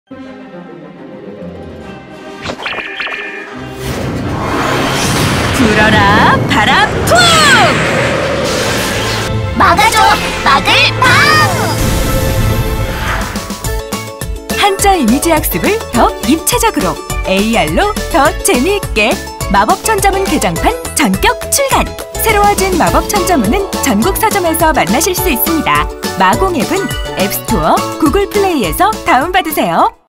[마법천자문 개정판] 1월 9일 출간* 한자를 직접 써보는 마공앱 AR 기능! 새로워진 마천을 만나보세요_30초 버전 l 마법천자문TV
[마법천자문 개정판] 2019년 1월 9일 출간!
AR로 업그레이드 된 마법천자문을
지금 서점에서, 마공앱에서 만나보세요!
**마공앱 다운로드 하고 이벤트도 참여하세요~